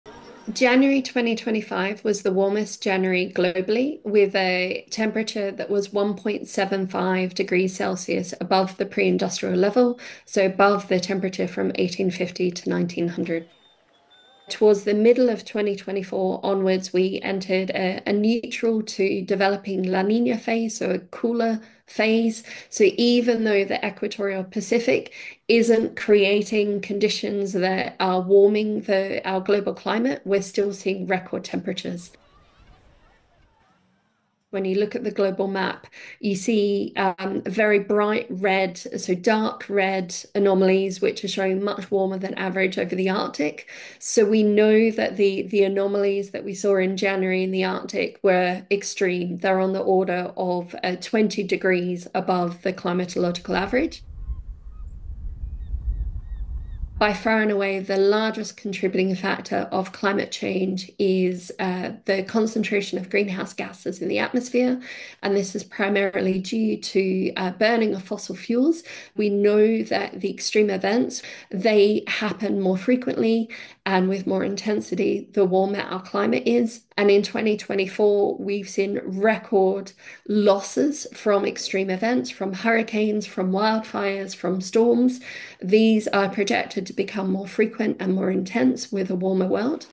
January #2025 was warmest January worldwide ever, according to reports
Earth continued to see record-setting temperatures in January 2025, according to reports. Concentration of greenhouse gases in the atmosphere is cited as a main reason for the rising temperatures.